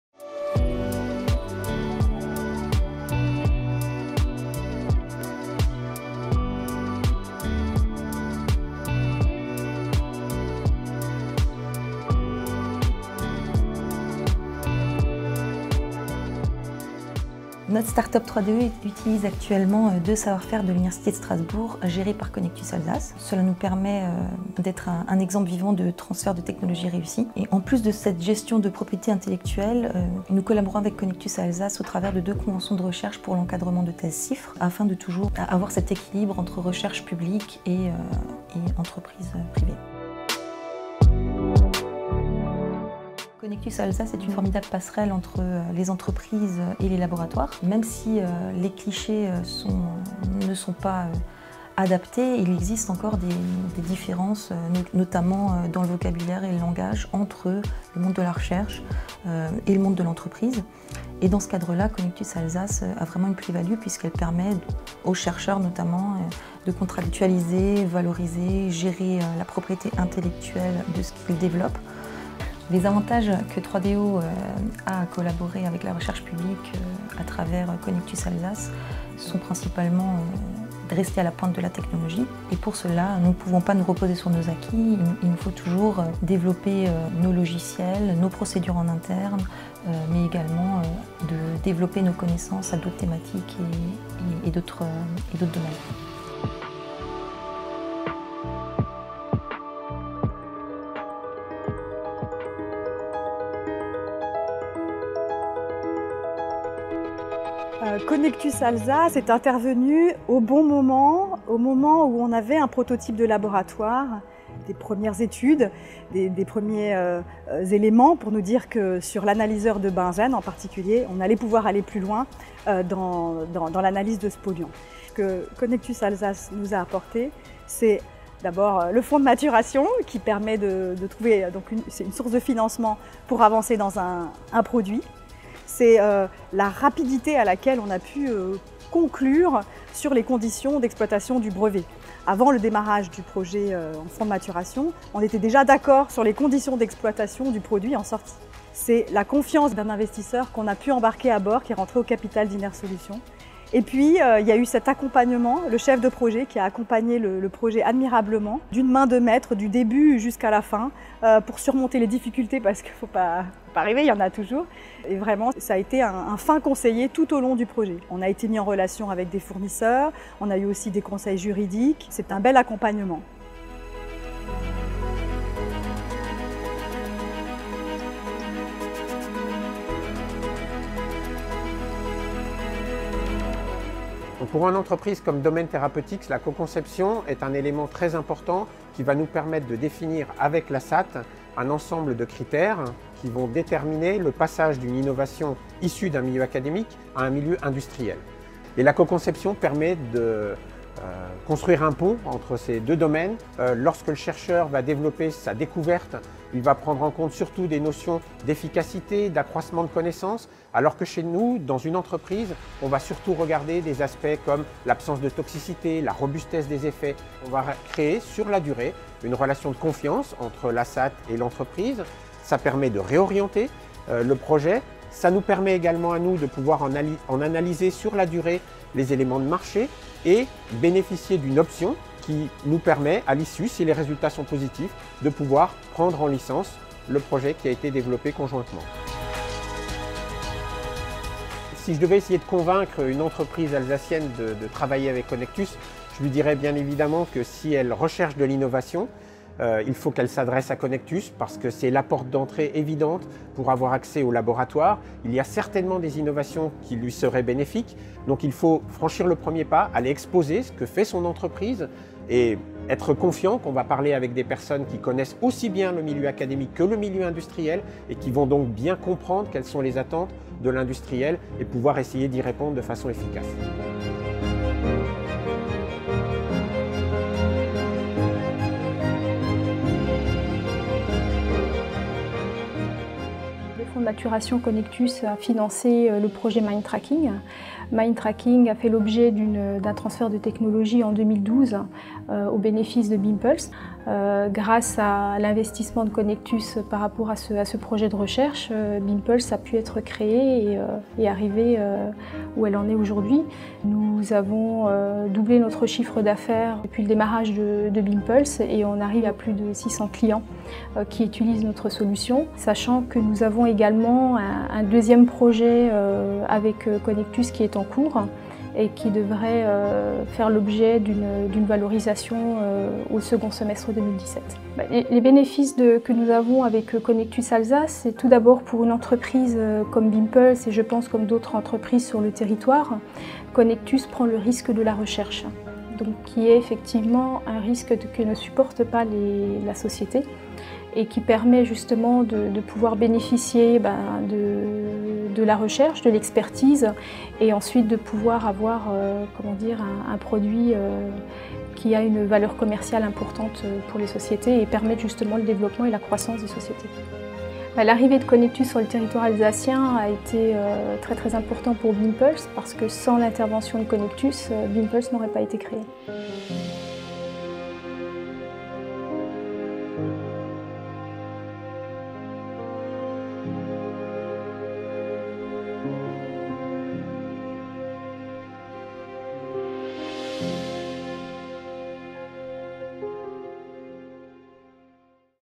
PAROLES DE 4 ENTREPRISES_par Conectus
Découvrez le témoignage de 4 entreprises qui ont été propulsées à partir d'innovations nées de la recherche publique et accompagnées par la SATT Conectus.